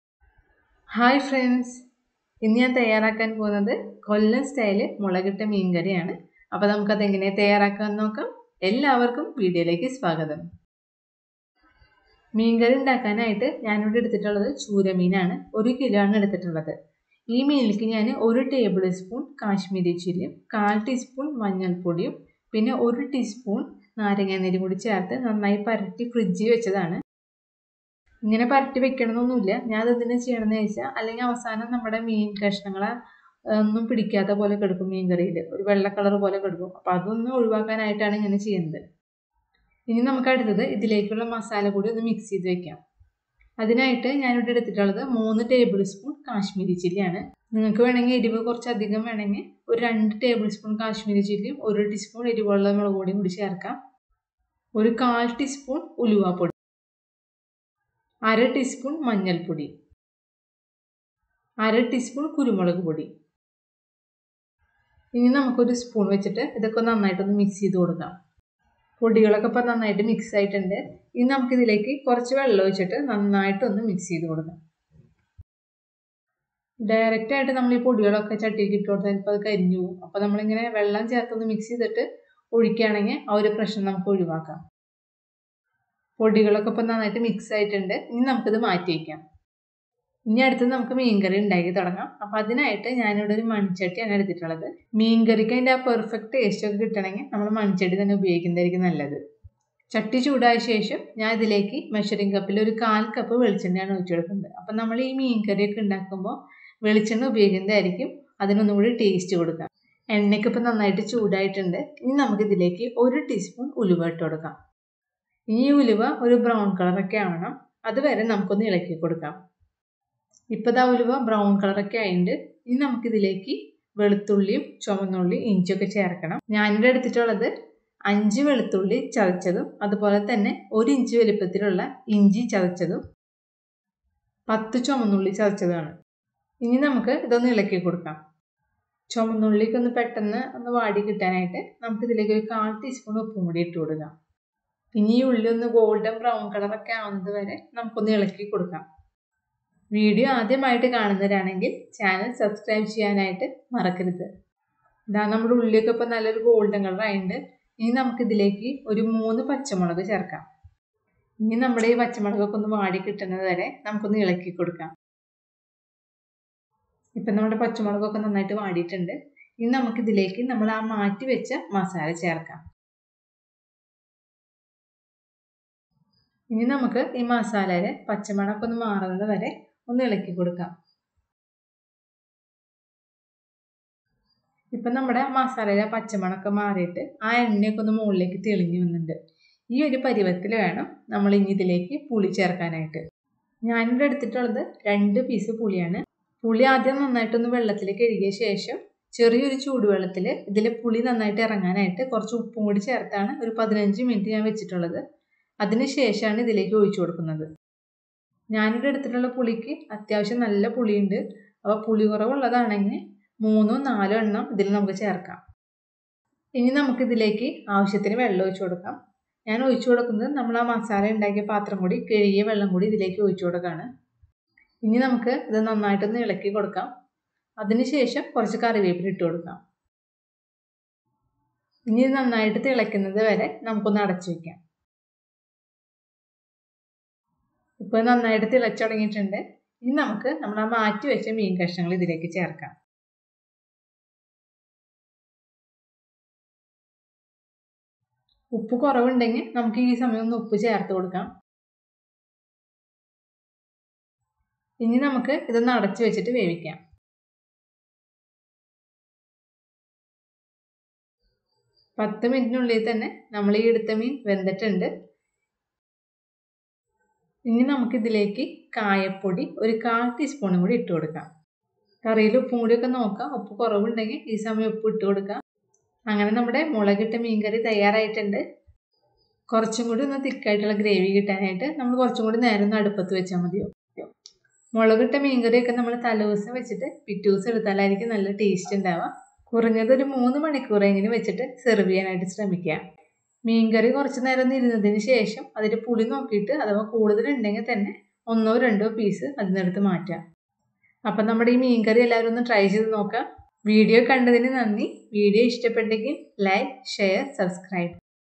കിടിലൻ രുചിയിൽ മീൻ കറി മുളകിട്ടത്/ Kollam Style Meen Curry/ Fish Curry Recipe
Ingredients
Fish – 1 Kg
Kashmiri chilli – 1 +3 tablespoon
Turmeric powder – ¼ + ½  teaspoon
Lime juice – 1 teaspoon
Fenugreek powder – ¼ teaspoon
Fenugreek – 1 teaspoon
Shallots – 10 Nos. (crushed)
Garlic – 5 Nos. (crushed)
Ginger – 1 inch size (crushed)
Salt – ¼ teaspoon
Chilly – 3 Nos.
Kudam puli – 2 to 3 pieces
Curry leaves
Kayam powder – ¼ teaspoon